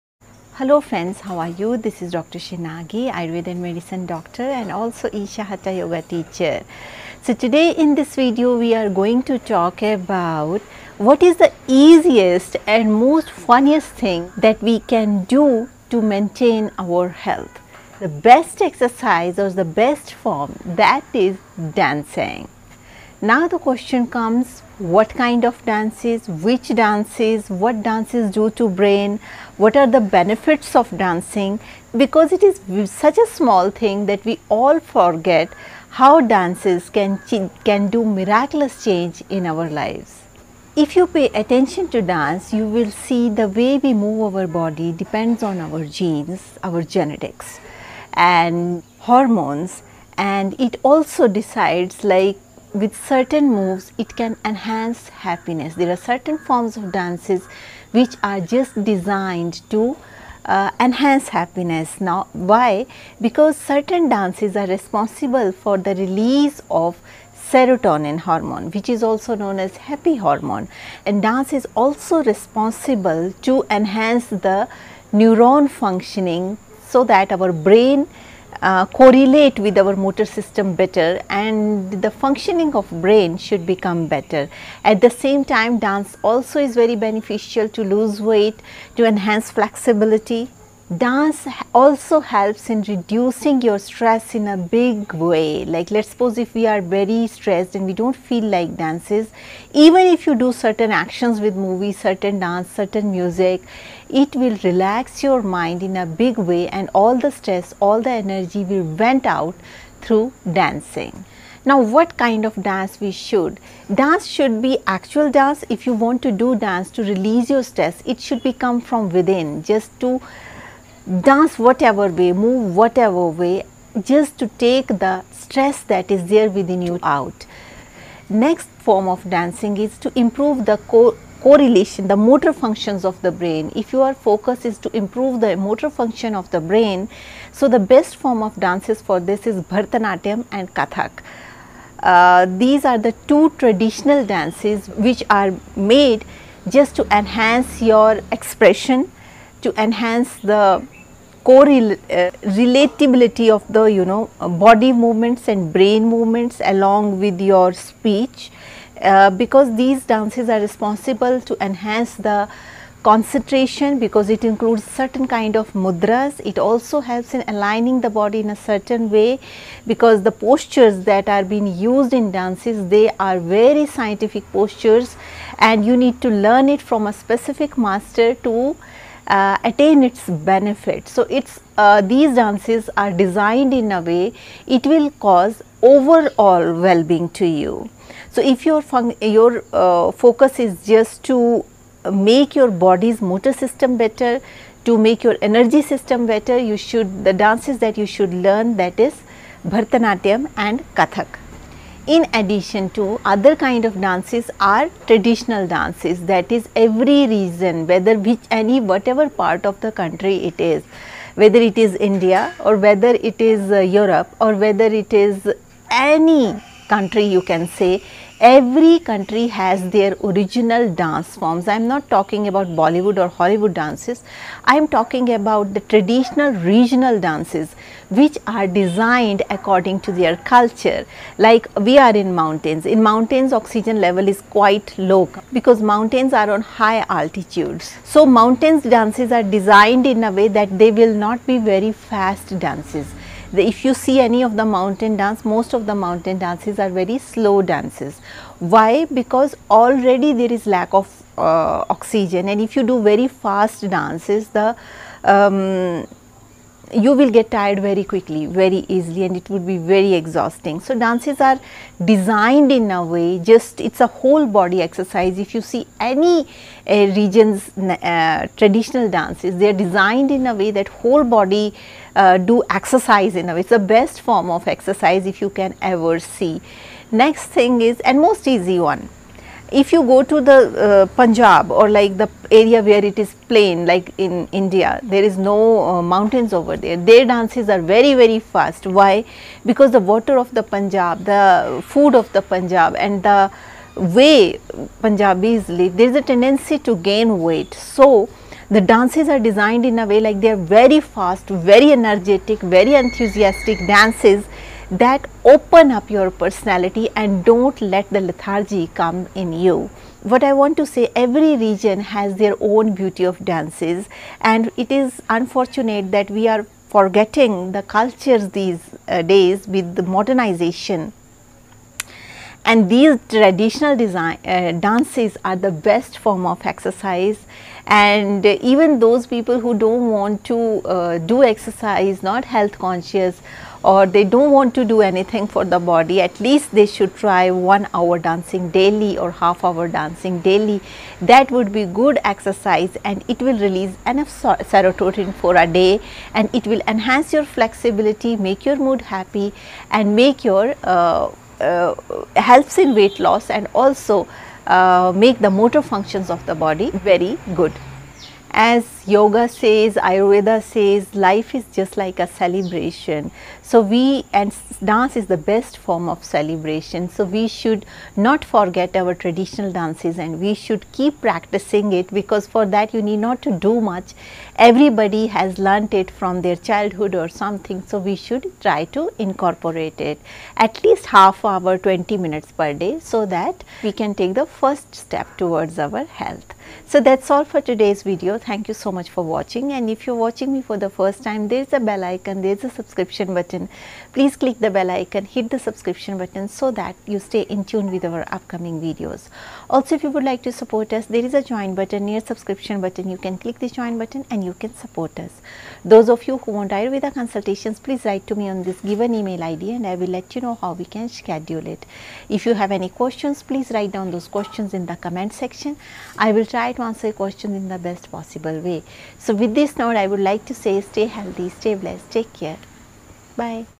Dancing a Easy way to fitness
@AyurvedaDoctorSriNagi  speaks about fitness with dance